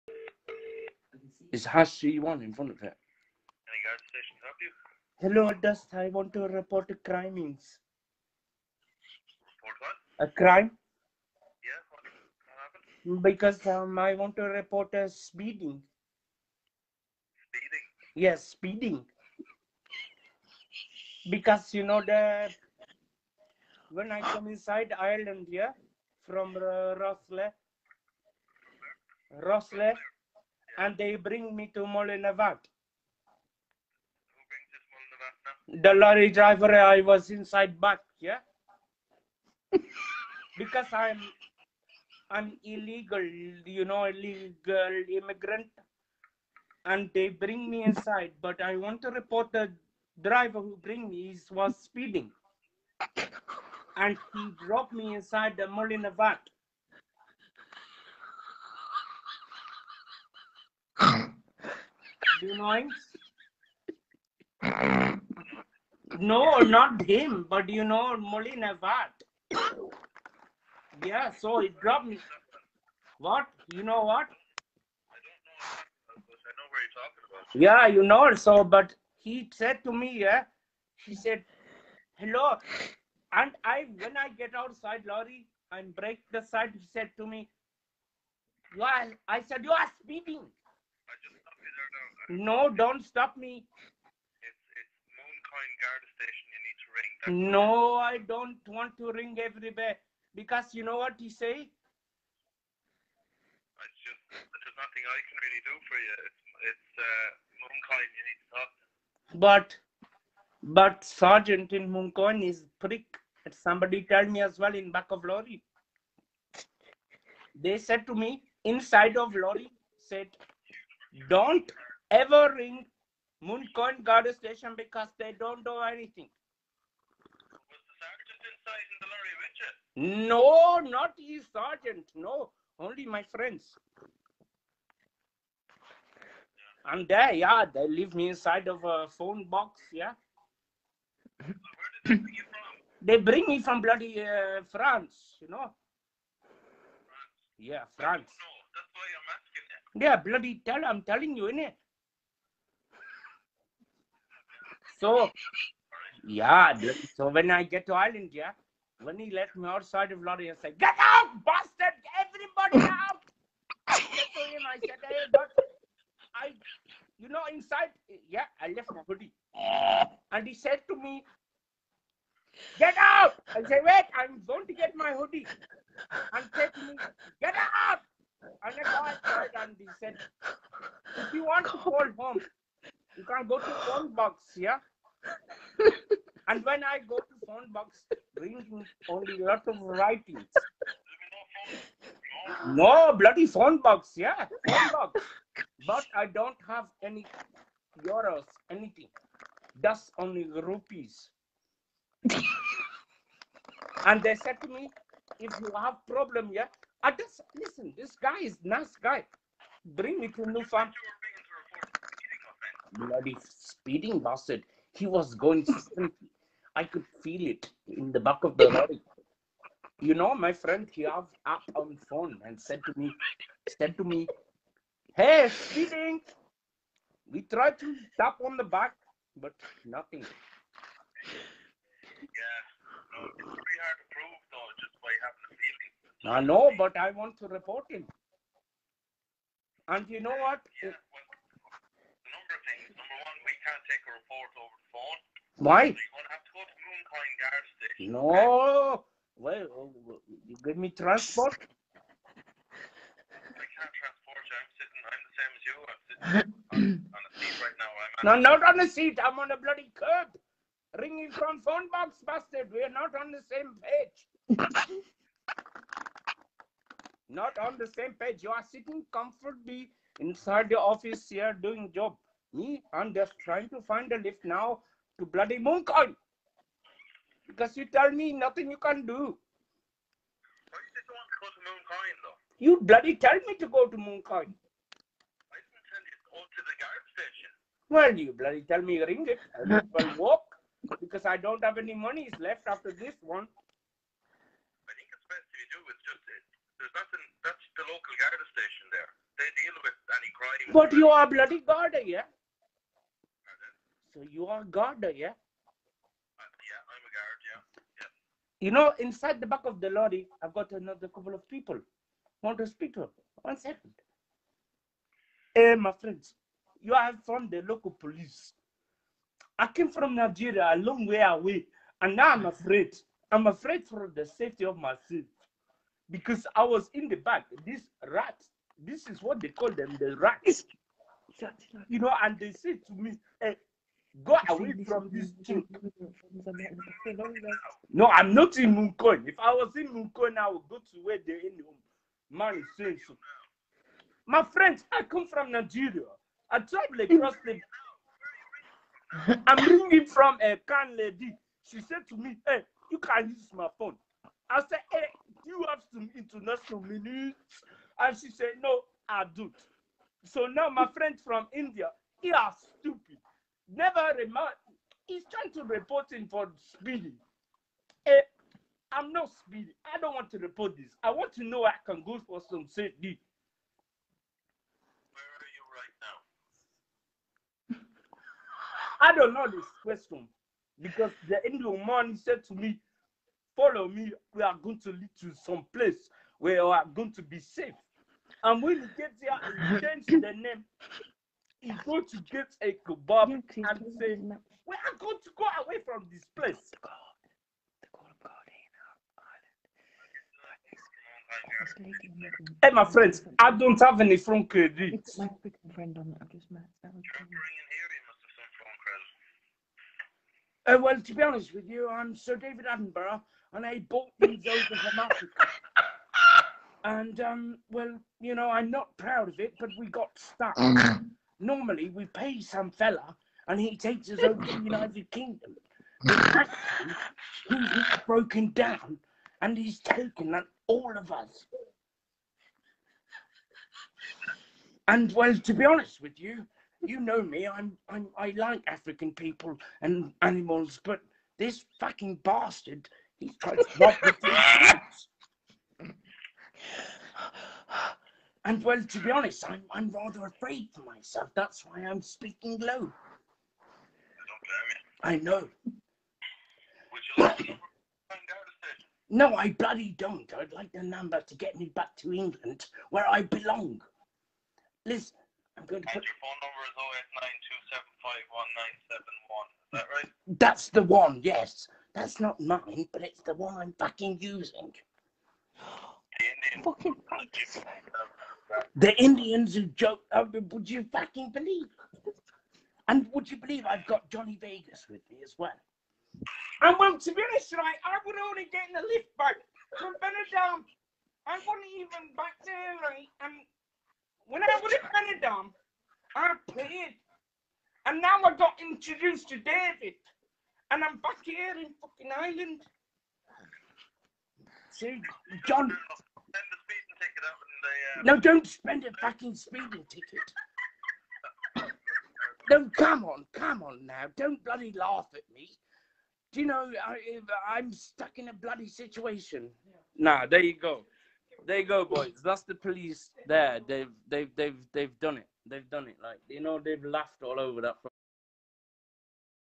My prank call to Irish Garda (police)
So I decided to report someone for speeding, Obviously with a twist better than Oliver